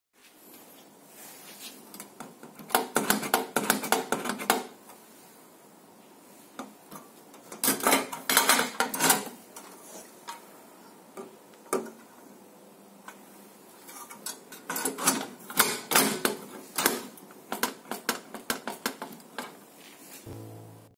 FIX: GOLI CTL Stove Burner is not stable, wiggling on dip pan
Issue: When the GOLI CTL stove burner is sitting on the drip pan, it wiggles.
Solution: Bend up prongs.